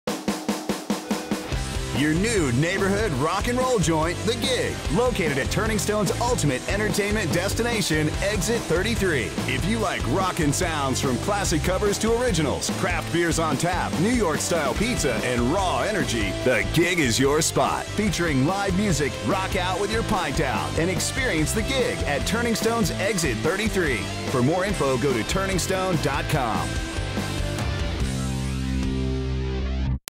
The Gig Rock & Roll Nightclub at Exit 33 | Turning Stone Resort Casino
Turning Stone Resort Casino | Verona, NY

Call us your neighborhood rock and roll joint. That's what we are. The Gig features local and regional bands, drink specials that won't empty your wallet, and rockin' sounds. That's how we roll. And it's not just music. The Gig serves up savory NY style pizza and cold craft beer on tap to back its tunes. So get ready to rock out with your cup out at The Gig.